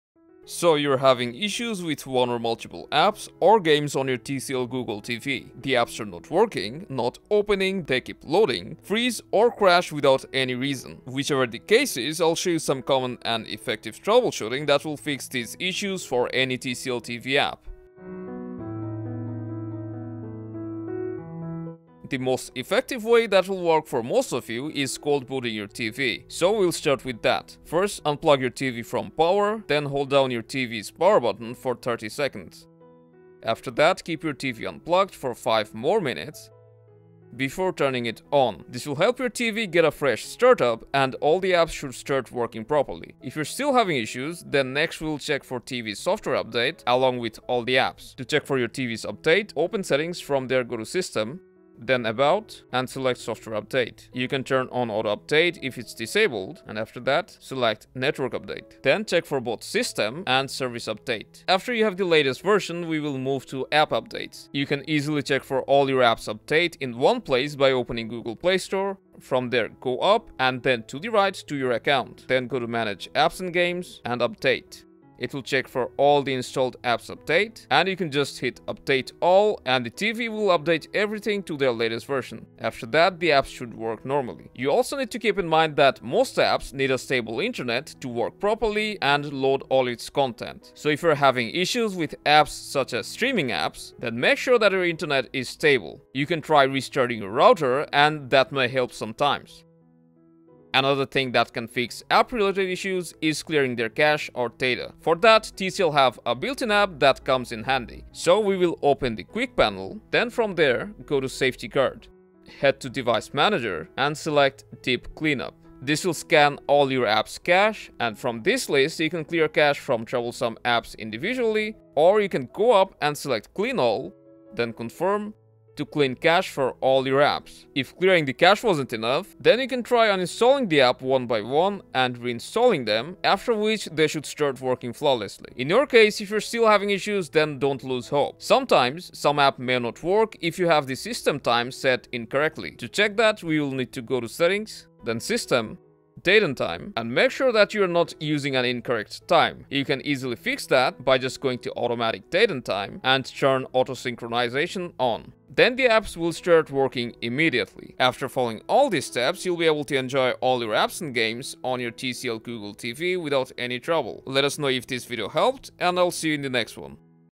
TCL Google TV: Apps Not Working? - How to Fix App Crashes/Not Loading or Opening!
Struggling with Apps not working on your TCL Smart Google TV and looking for the best ways to fix the issue? Are you facing various app problems on your TCL Google TV, like-

* Apps not opening,
* Apps not loading,
* Apps immediately close after launching,
* Apps keep crashing at random,
* Streaming Apps not working,
* App crashes during launch,

Don’t panic, in this video, the Fix369 Team has brought you 5 Simple and swift solutions to fix Apps not working on TCL Google Smart TV effectively.

0:00 Apps not working on TCL Google TV?
0:22 Solution 1: Cold boot your TV
0:43 Solution 2: Check for Software updates and update all apps
1:24 Solution 3: Check and fix the Internet connection
1:41 Solution 4: Clear the App Cache to free up storage and Reinstall the Problematic app
2:18 Solution 5: Set the Correct Date and Time